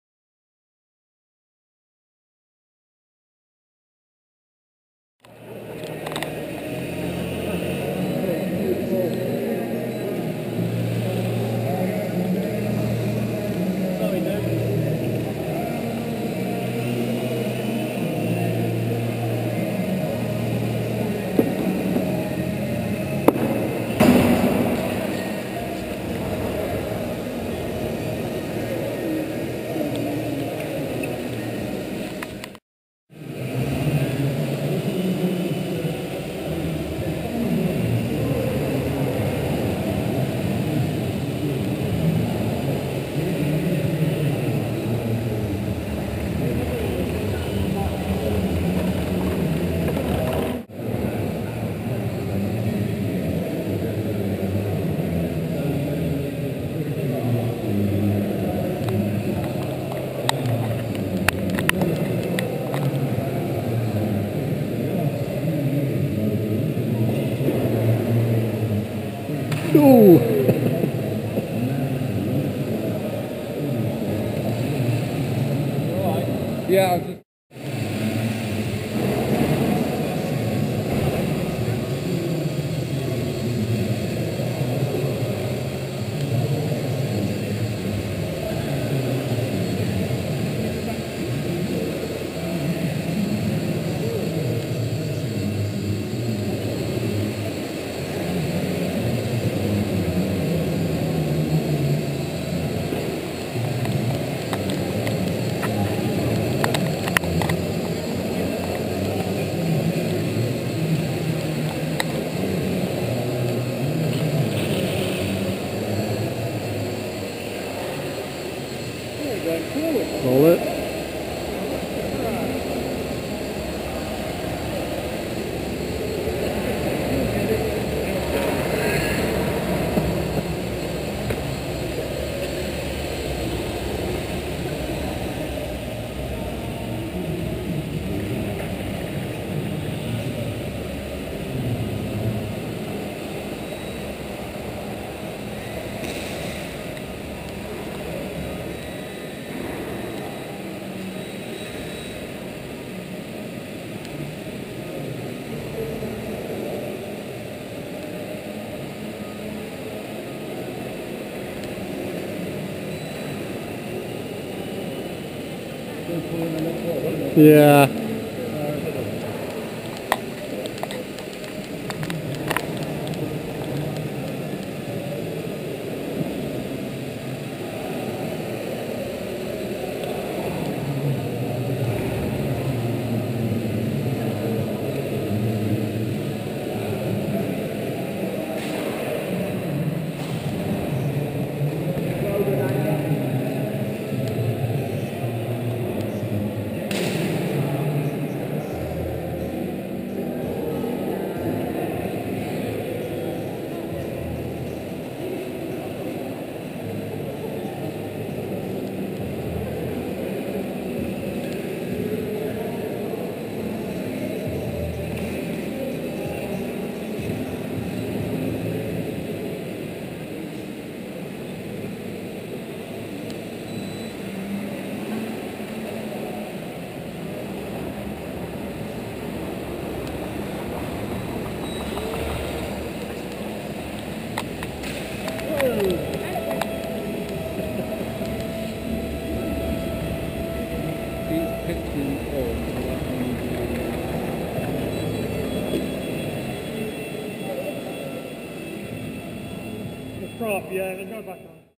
Hobbyzone Champ at LMAC 23.2.2013 GoPro HD 2
Hobbyzone Champ
Leicester Model Aero Club fly-in
Parklands Wigston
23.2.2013
filmed on a GoPro HD2
HD
Champ+DX6i